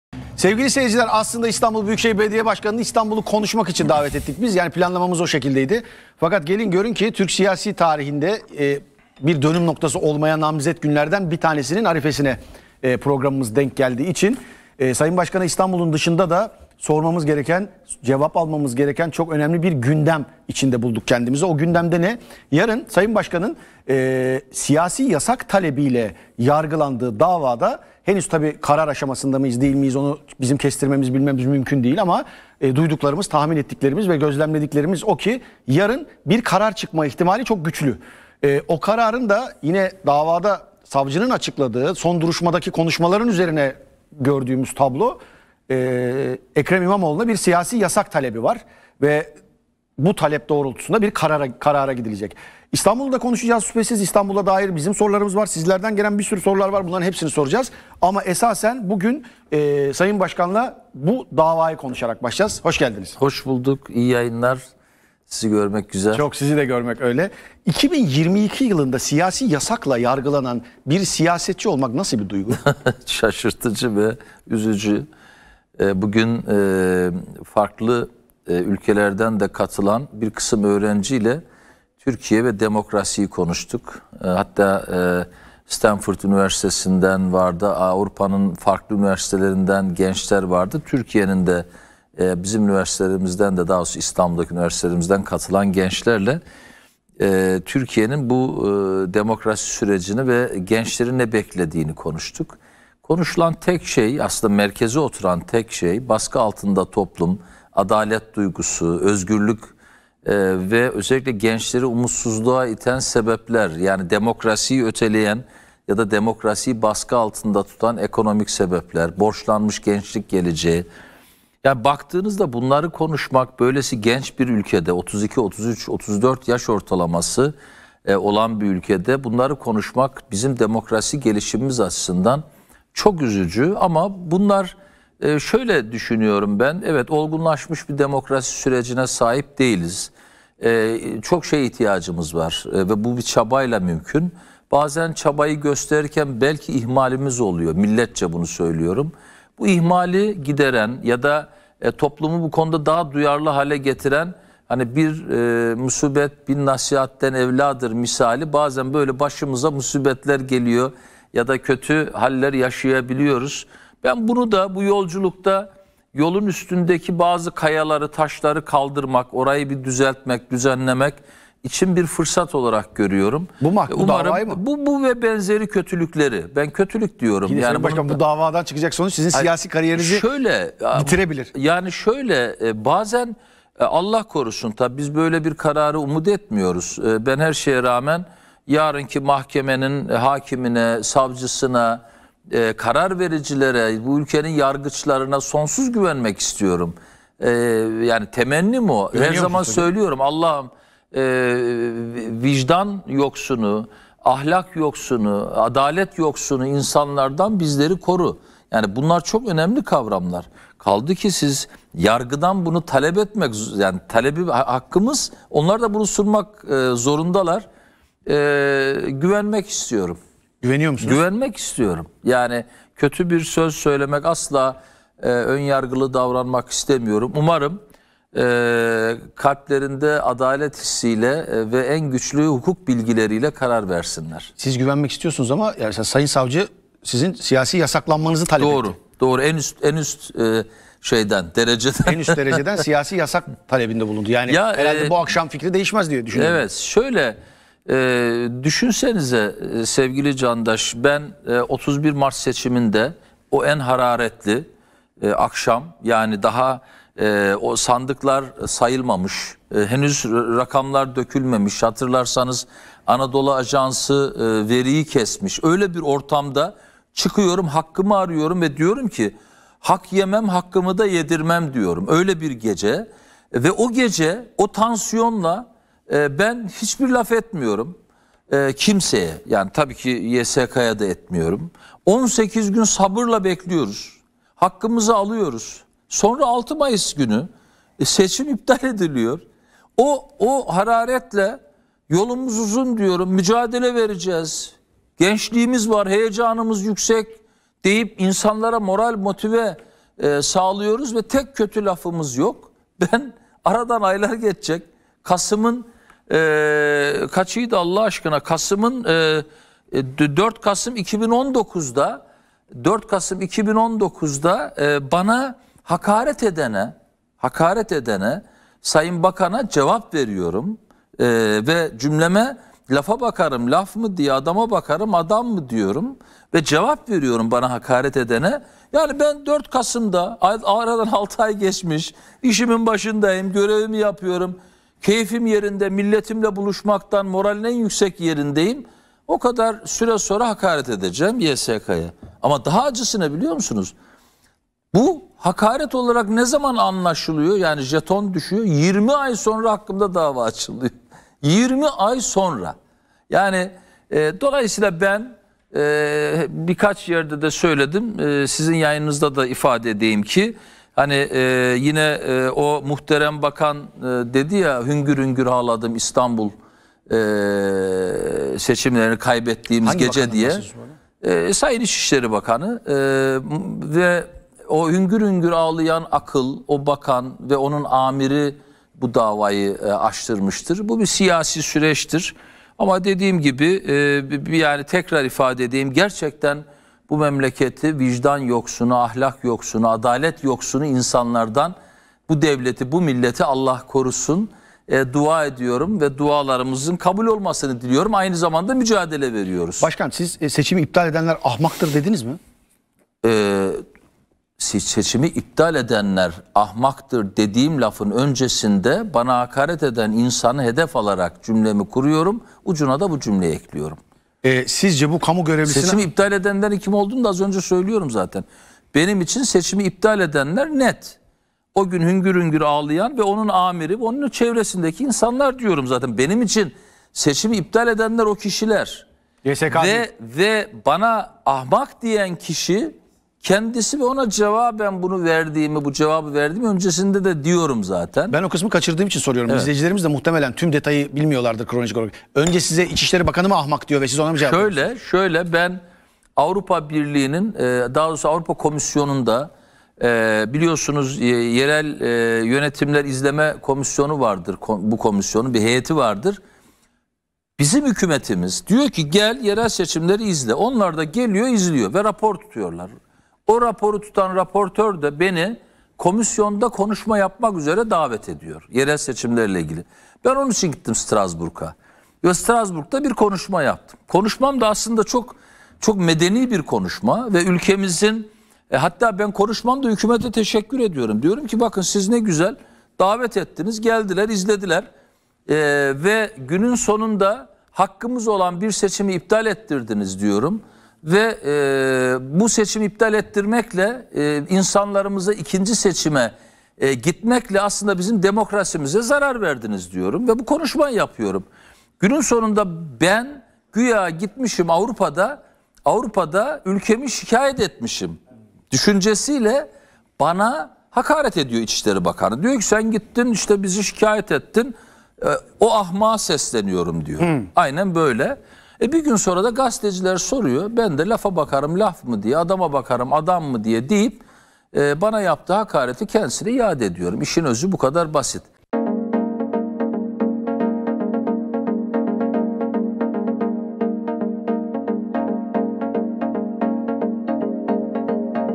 Ekrem İmamoğlu'na Siyasi Yasak Çıkarsa Ne Olacak? | Candaş Tolga Işık ile Az Önce Konuştum
Ekrem İmamoğlu'ndan kritik açıklamalar

İşte tv100'ü seyredebileceğiniz tüm platformlar

Kablo tv: 37. kanal
Digitürk: 37. kanal
D-Smart: 37.kanal
Turkcell TV+: 37. kanal
Tivibu: 47. Kanal